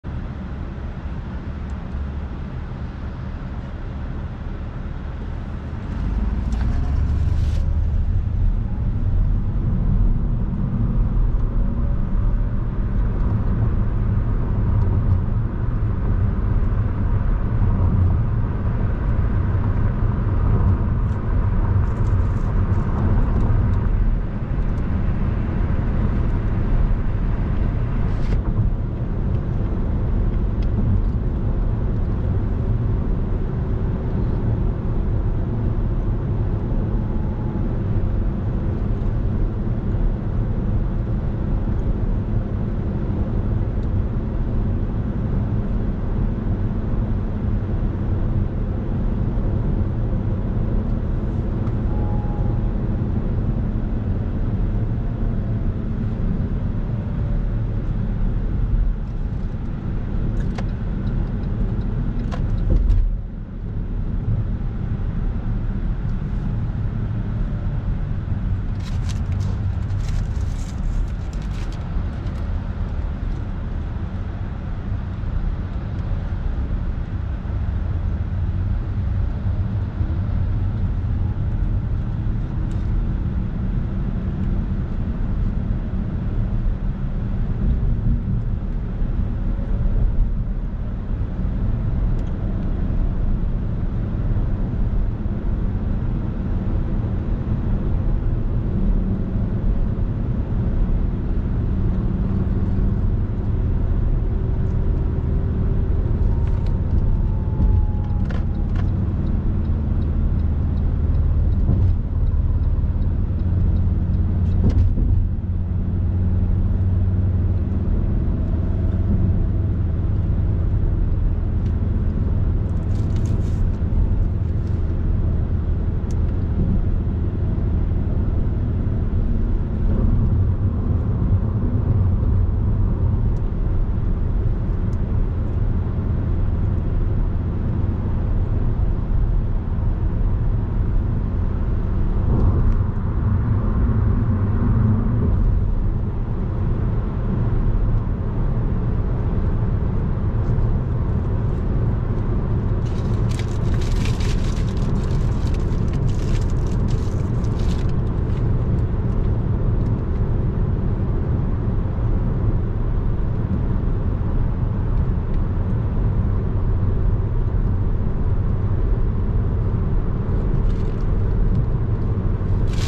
Eating Bacon Egg & Cheese Burrito from Rili-B's on John Wayne Parkway, Maricopa, Arizona, NGH23670
Having a Breakfast Burrito on AZ-347 North, 31 May 2024,
Rili-B's Taco Shop, 19975 N John Wayne Pkwy, Maricopa, AZ, 85138,
Eating Bacon Egg & Cheese Burrito from Rili-B's Taco Shop on John Wayne Parkway, Maricopa, Arizona, NGH23670